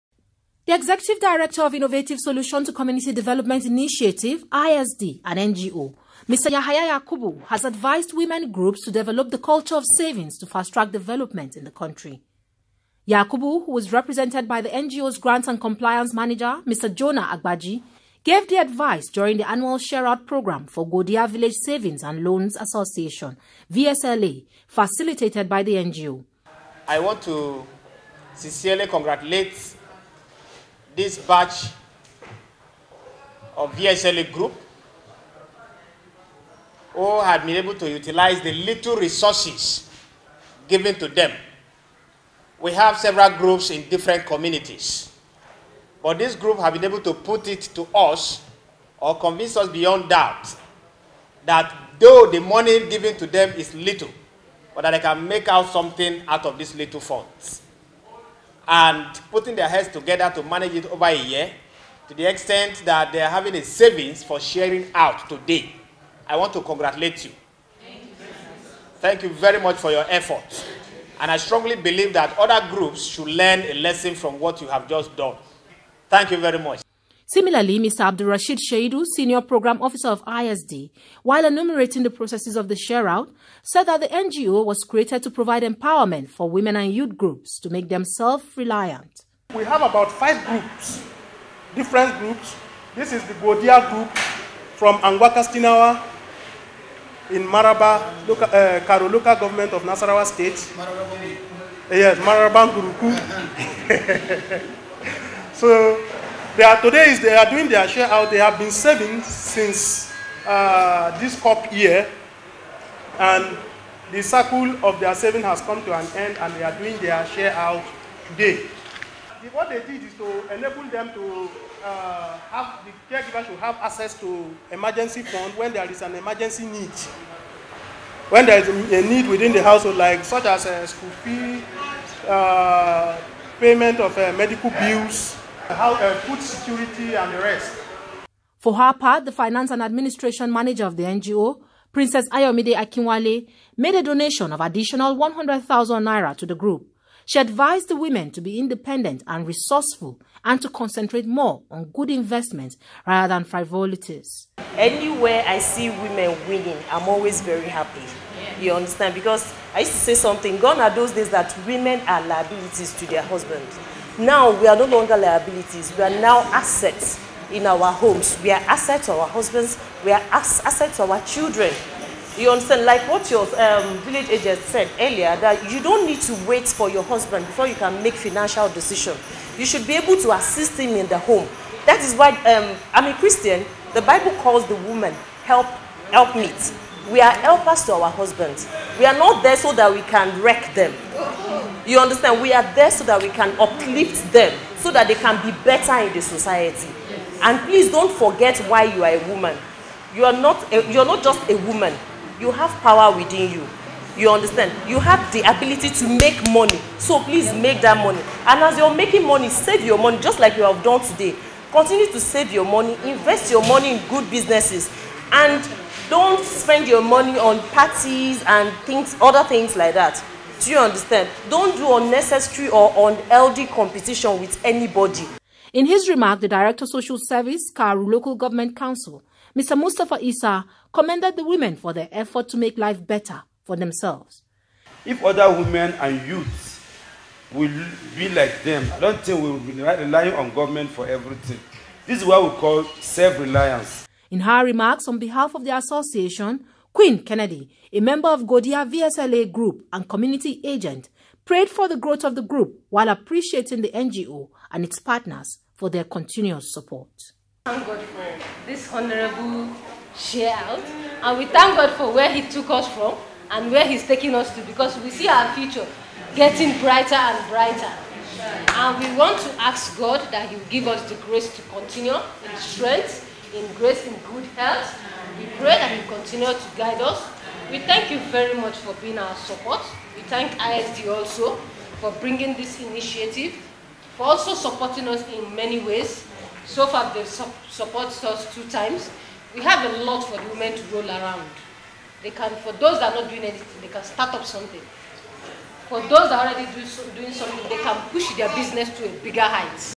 NGO urges women, youth groups to develop savings culture
The Executive Director of Innovative Solution to community Development initiative (ISD), an NGO, Mr Yahaya Yakubu has advised women groups to develop the culture of savings to fasttrack development in the country